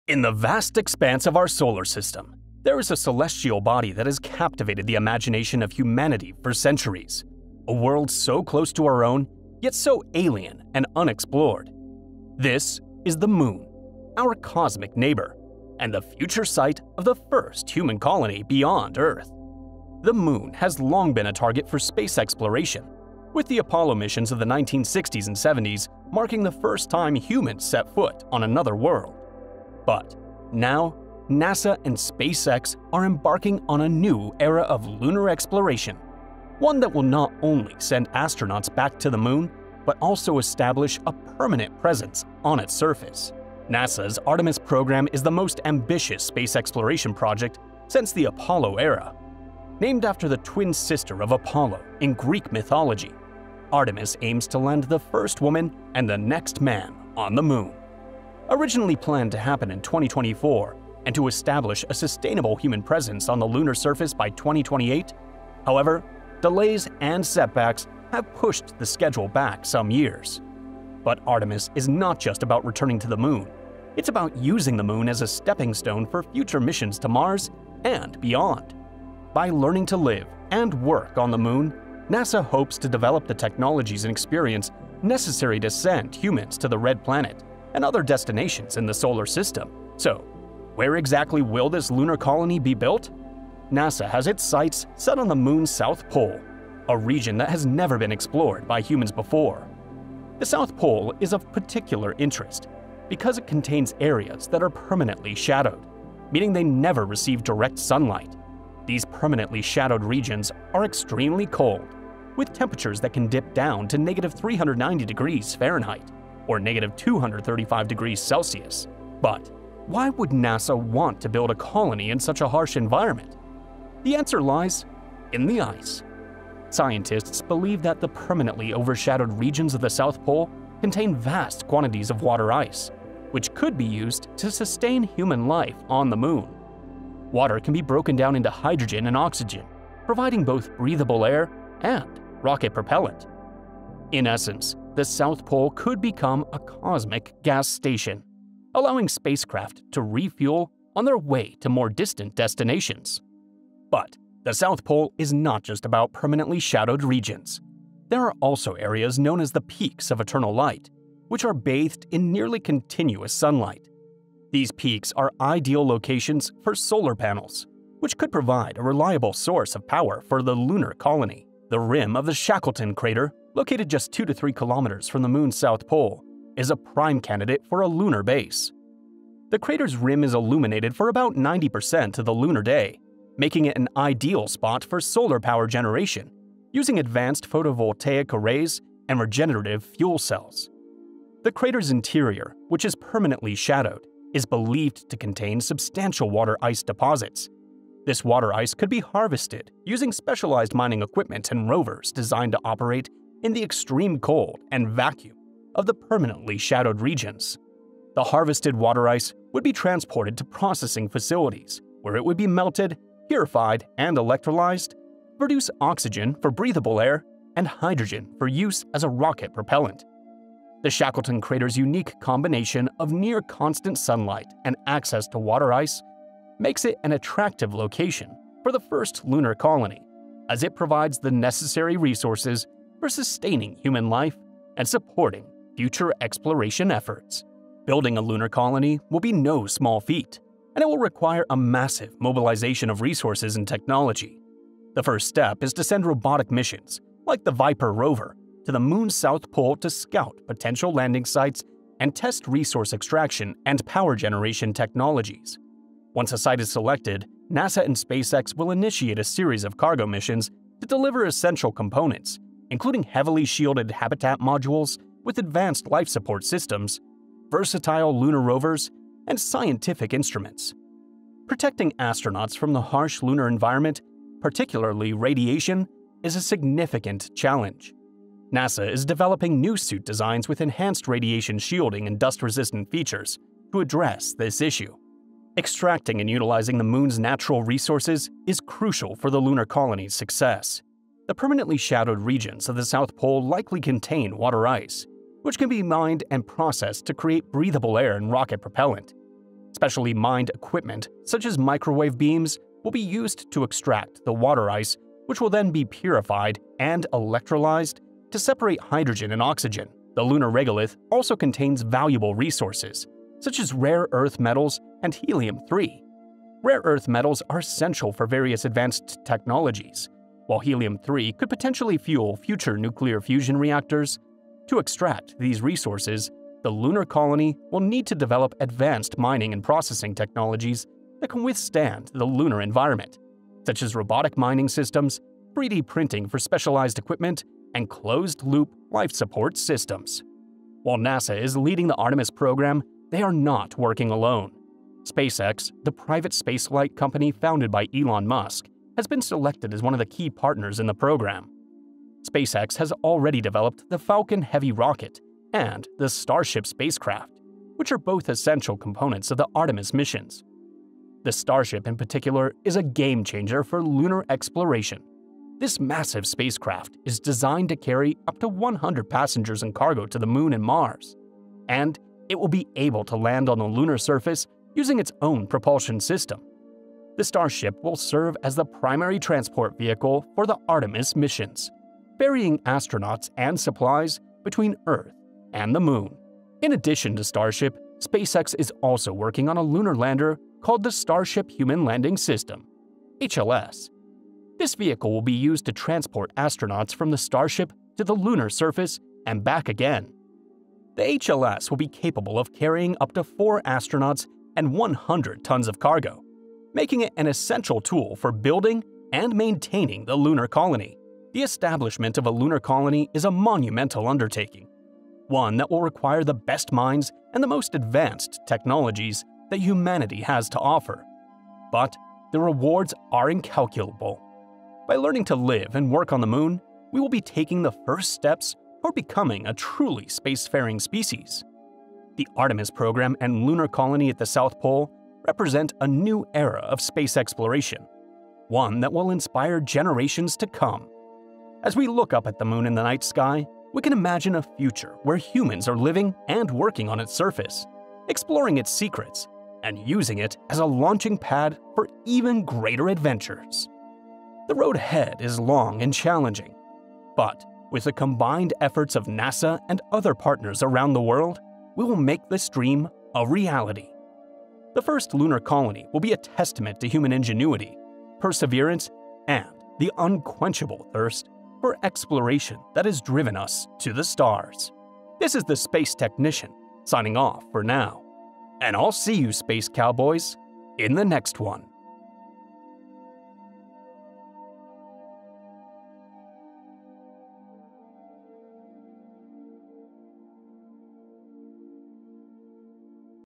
This Is Where NASA and SpaceX Will Build Their MOON Base!
🚀🌕 Join us as NASA and SpaceX gear up to build their groundbreaking MOON Base! Discover the vast, icy plains of the lunar South Pole, where humanity will soon mark its territory beyond Earth. 🌌

In this thrilling video, your specialist, The Space Technician, takes you on an epic journey to where the first lunar colony will rise. Nestled in the Shackleton Crater's rim, this site offers nearly constant sunlight and access to essential water ice, setting the stage for a sustainable human presence by 2028. 🌑💧

We delve into the wonders of the Artemis program, aiming to propel astronauts into orbit and beyond, using advanced technologies like the Starship and Dream Chaser. Witness how these space cargo transportation marvels will help establish the Moon as a cosmic outpost for future Mars missions. 🚀🛰️

Marvel at the ingenuity of using the Moon's resources, where robotic mining systems and 3D printing will transform raw lunar regolith into life-sustaining elements. The blend of cutting-edge technology and human courage could open new realms of possibilities in space exploration. 🌍➡️🪐

Thank you, Spacers, for joining this journey with us! And I'll be seeing you on the next one! 🌟👩‍🚀👨‍🚀

CHAPTERS:
0:00 - Moon Exploration Overview
0:41 - NASA Artemis Program Insights
1:30 - Moon South Pole Features
3:58 - Lunar Colony Construction
5:41 - SpaceX Collaboration in Artemis
6:51 - Future of Lunar Exploration